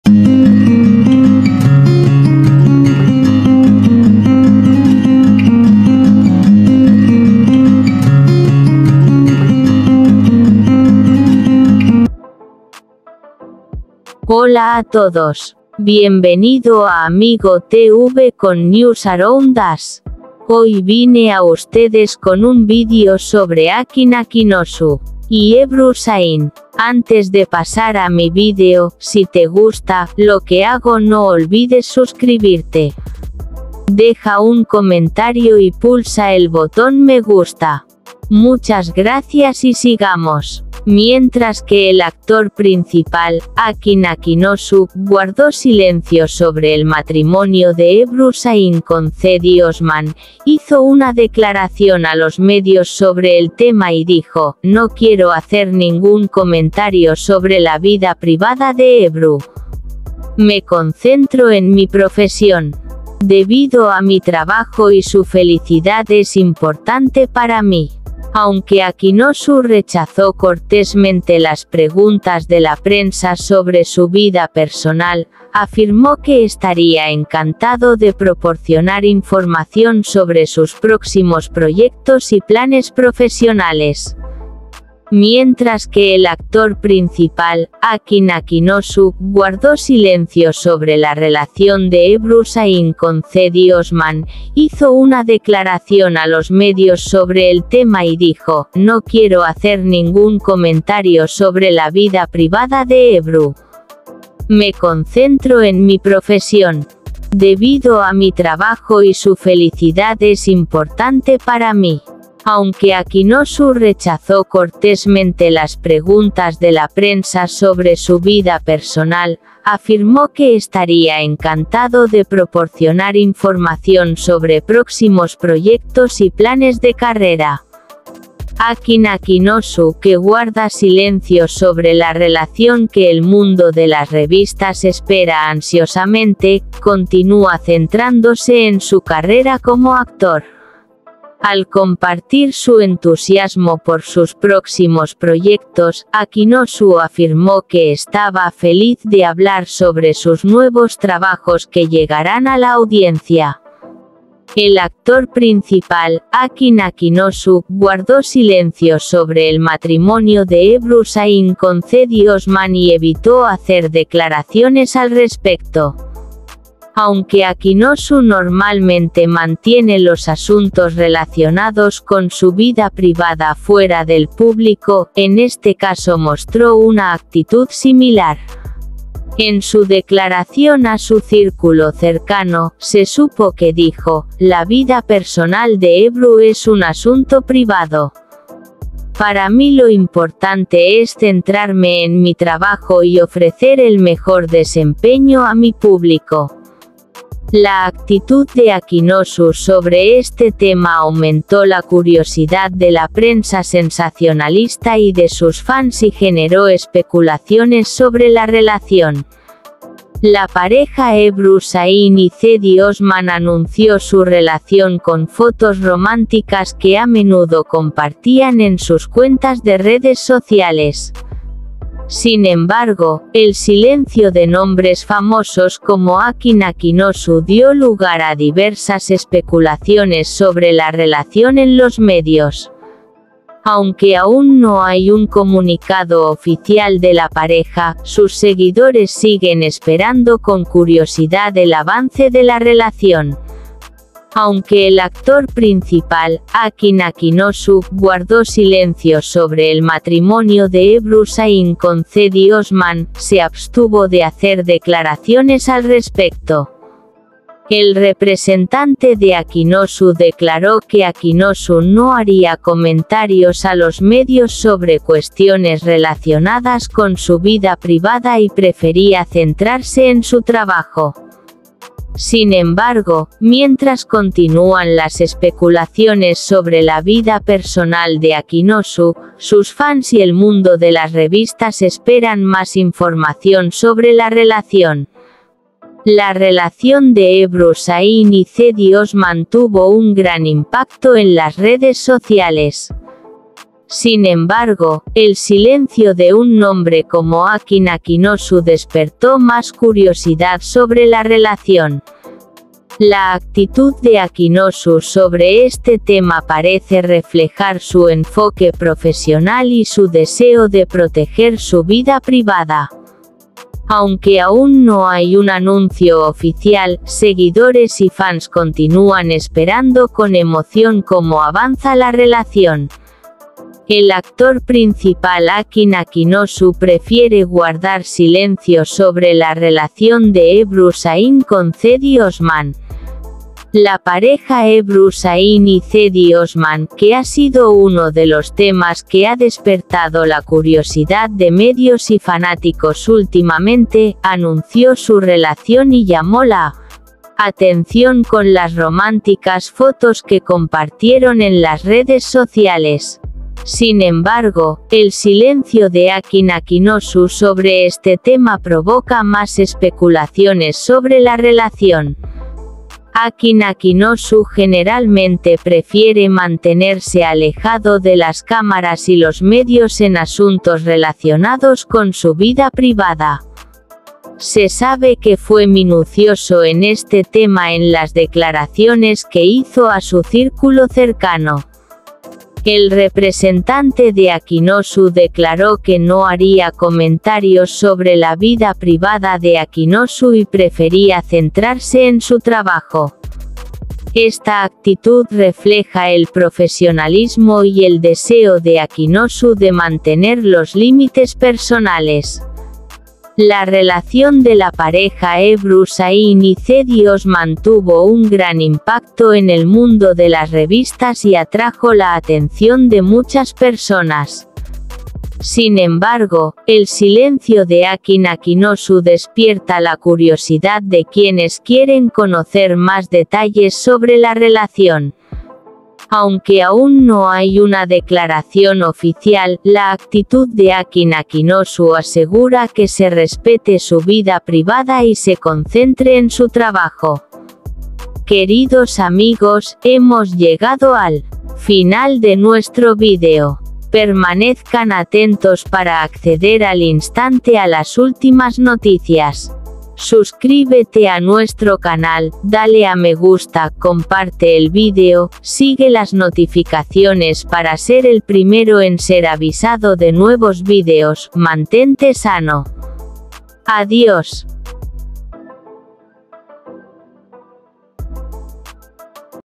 "Akın Akınözü permanece en silencio: Ebru Şahin y Cedi Osman no hablaron de su relación"
"Akın Akınözü permanece en silencio: Ebru Şahin y Cedi Osman no hablaron de su relación" @Amigotv103